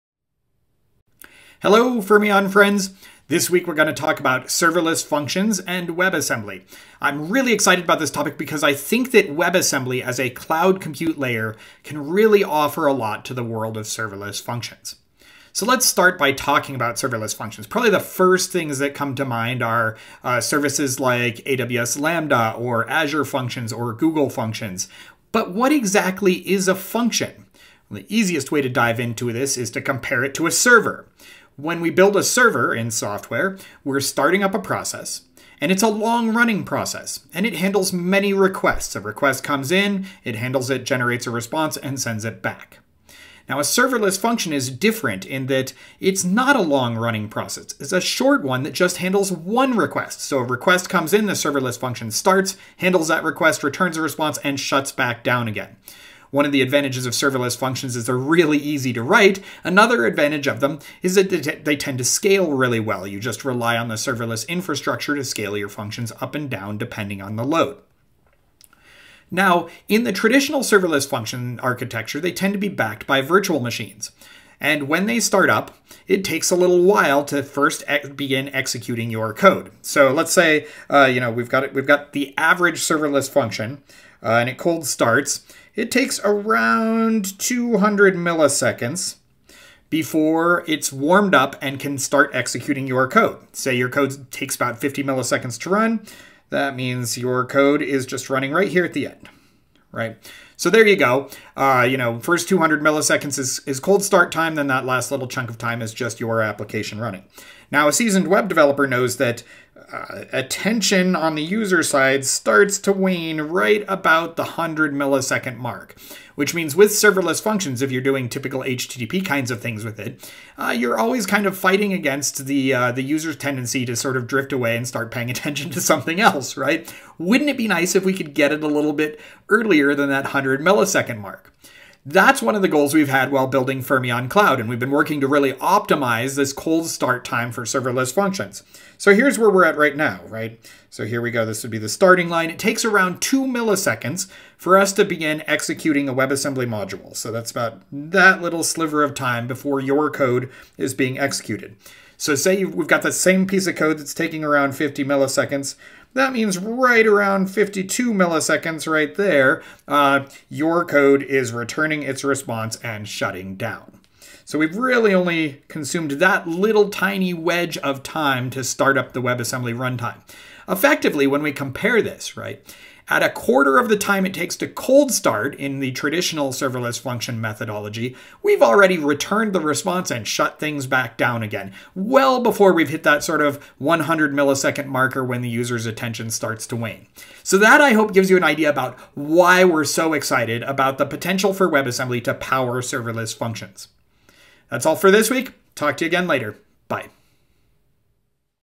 Creating Severless Functions with WebAssembly
With fast startup times and a strong security model, WebAssembly aka Wasm is the perfect technology for serverless functions.

Join Matt Butcher (CEO of Fermyon) where he explains what serverless functions are, and how WebAssembly really amps up the speed.